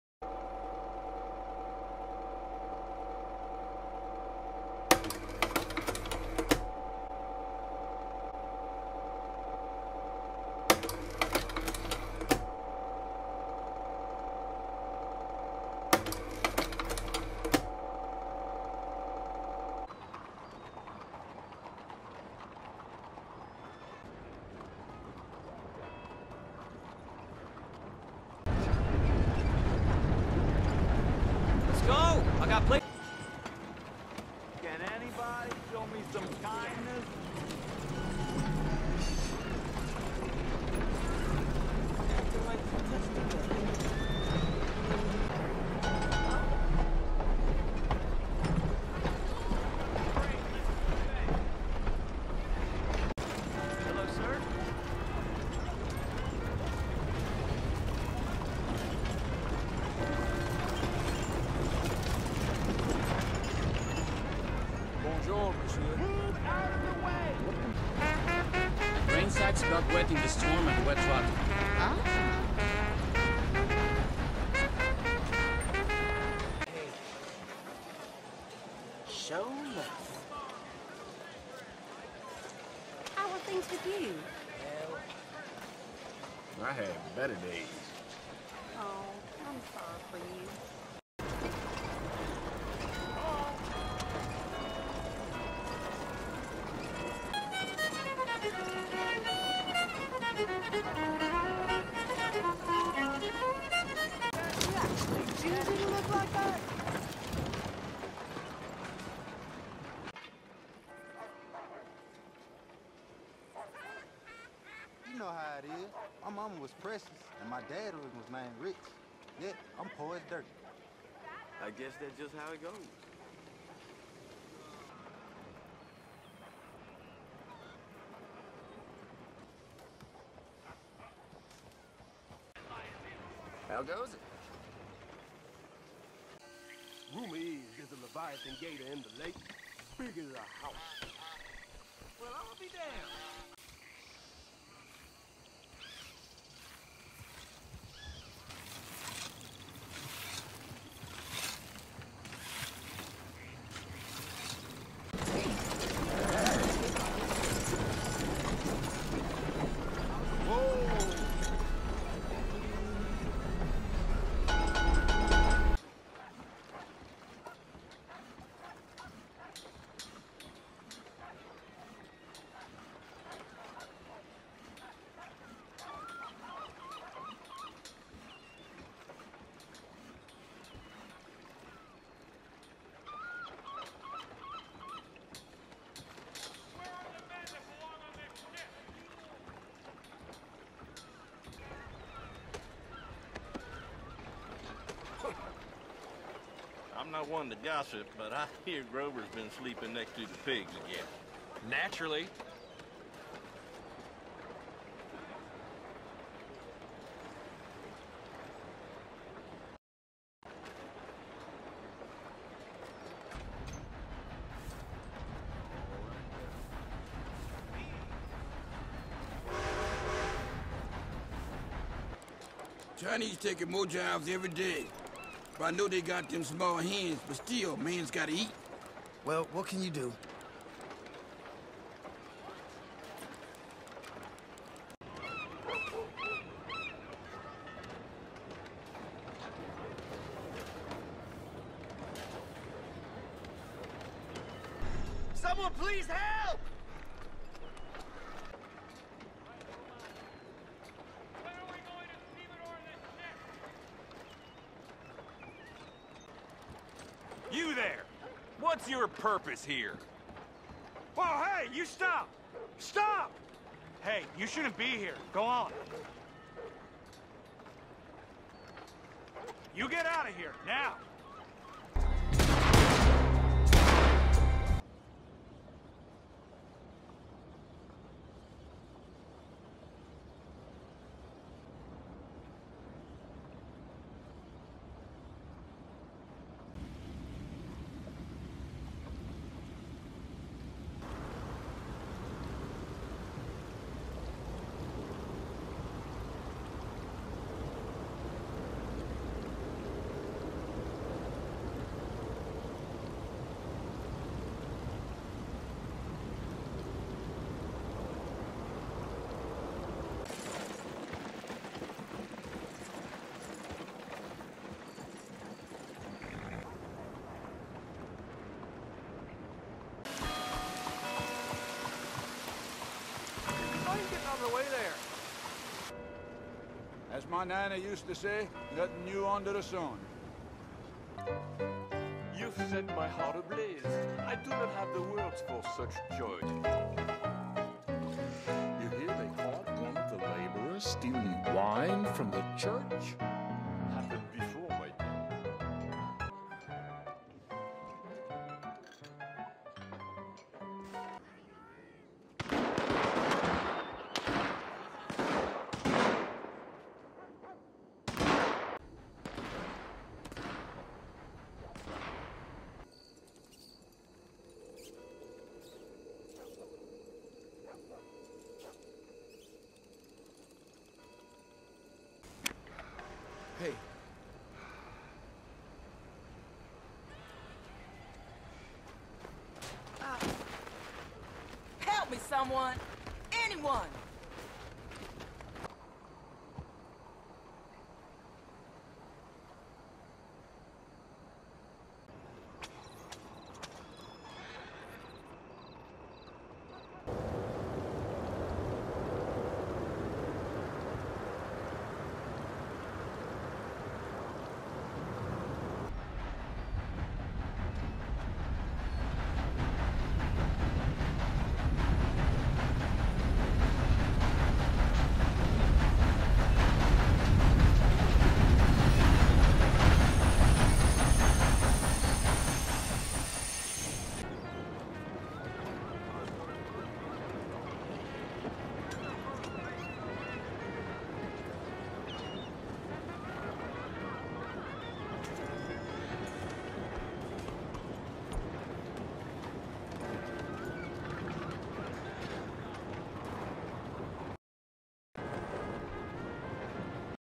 Saint Denis Shot Test 1898
An ever evolving industrial city under the shadow of smog, class disparity, crime, and corruption. Yet through the muck, there is beauty to be found. The following is an unscripted every day look into what goes on in the city of San Denis.